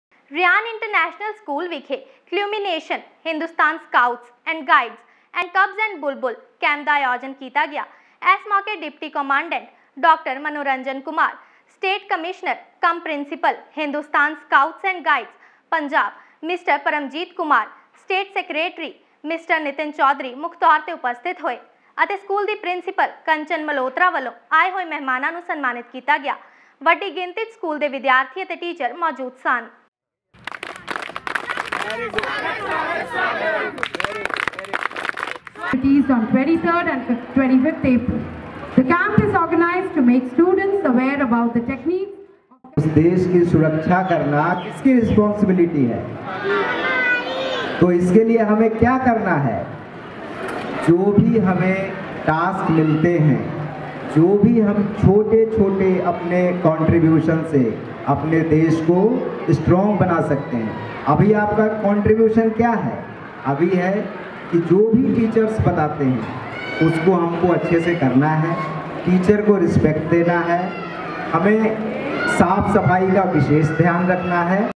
Cub and Bulbul Training Camp at Ryan international school Amritsar Organised By:- Hindustan Scouts
Video from Nitin Chaudhary